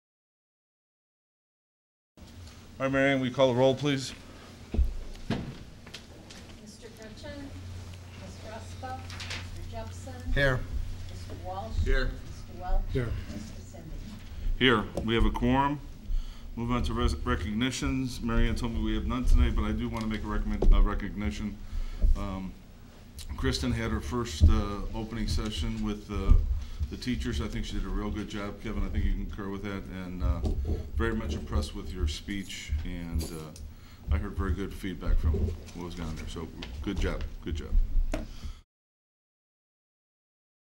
Recognitions 081214
Part 1 of the August 2014 regular business meeting of the Township High School District 208 Board of Education of Cook County in Riverside, Illinois.